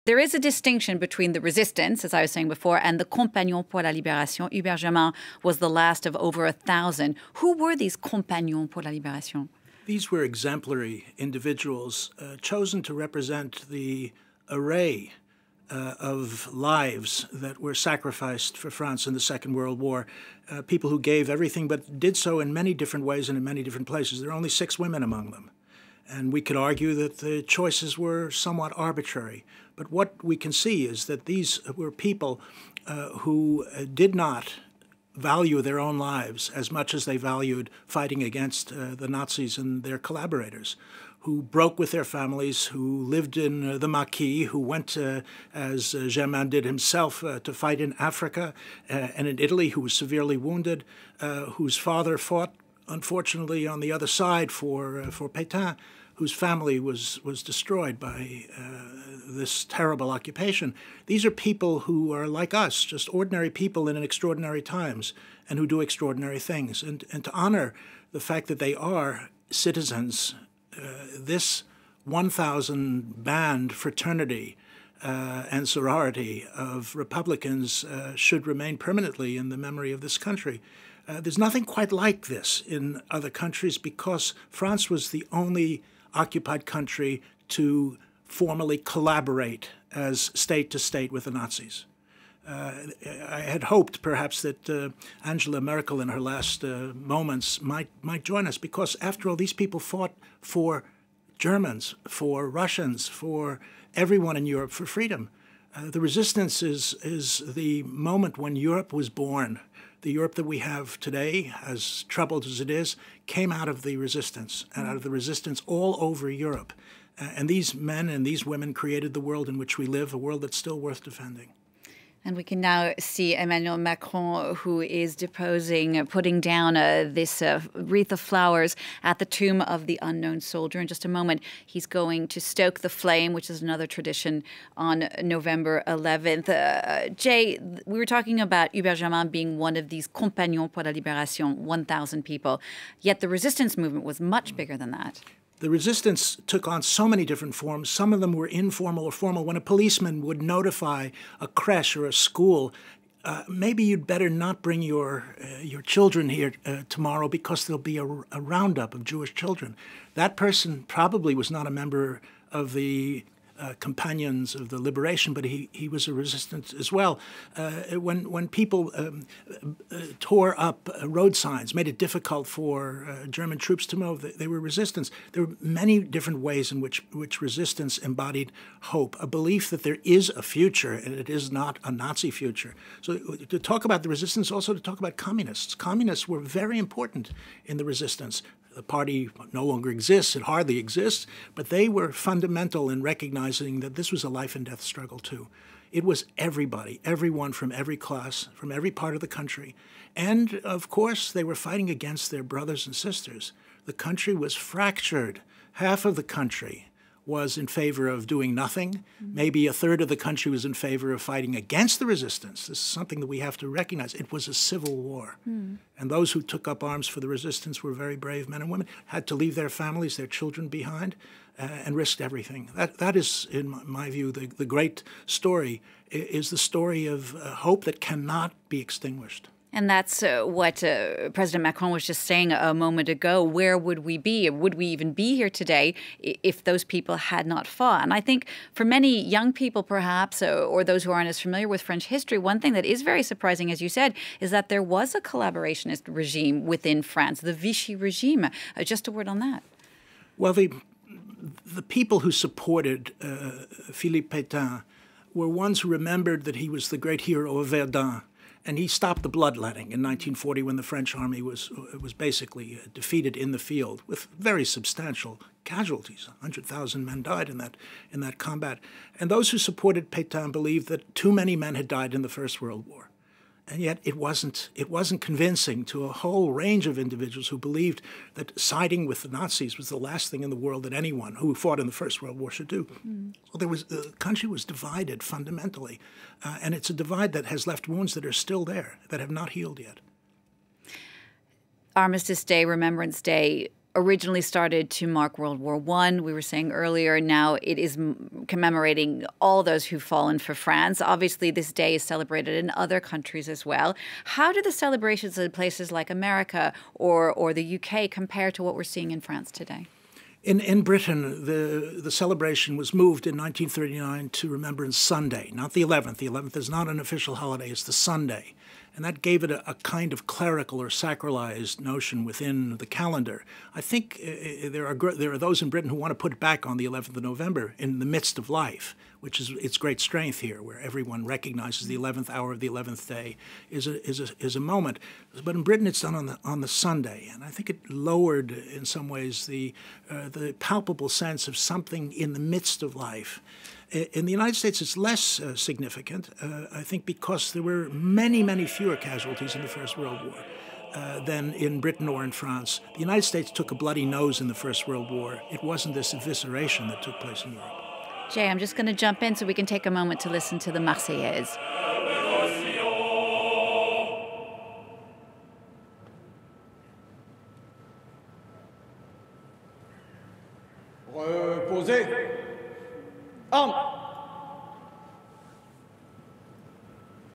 France : Who were the Companions of the Liberation ? • FRANCE 24 English
President Emmanuel Macron led tributes on Thursday to the last French Resistance fighter from World War II who died last month. Hubert Germain died aged 101 in October. The Paris-born fighter was one of 1,038 people decorated with the Order of the Liberation for their heroism by de Gaulle. Who were the "Compagnons de la Libération" (Companions of the Liberation) ? Jay Winter, professor of History Emeritus at Yale University, gives his analysis.